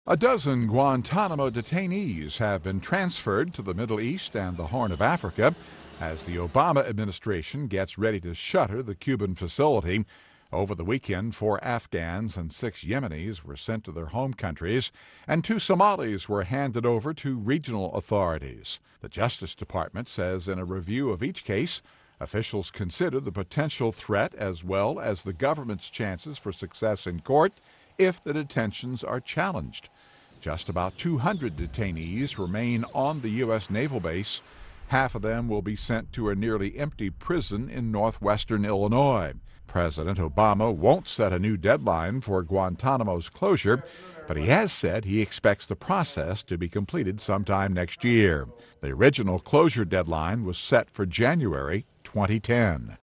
U.S. Sends 12 Gitmo Detainees to Their Home Nations
The U.S. has transferred a dozen Guantanamo detainees to Afghanistan, Yemen and the Somaliland region as the Obama administration continues to move captives out of the Cuban facility in preparation for its closure. (Dec. 20)